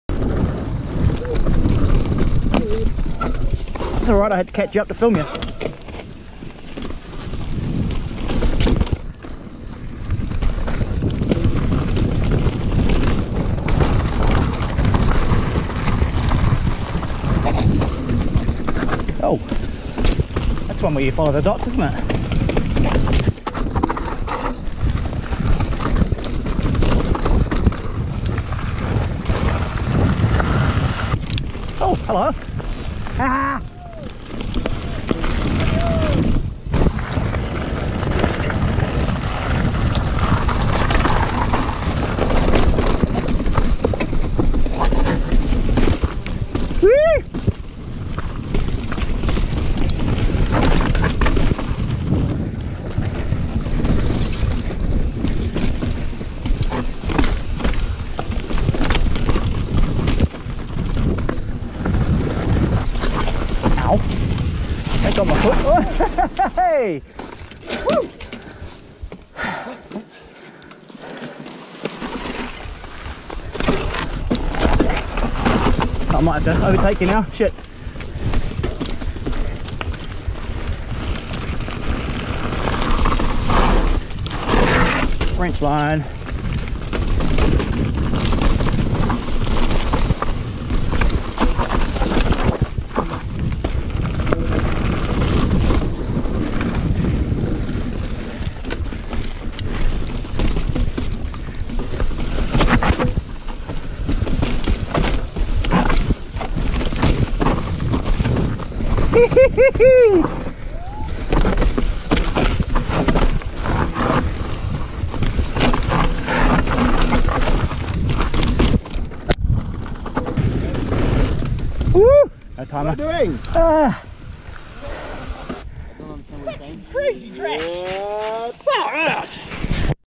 TransNZ Enduro Day 5 Stage 5 - Alexandra - Hazard County
Mixed back in with the riders for these last two stages. Hard to pick a favourite for the day, but this was pretty bloody good.

Apologies to Garth...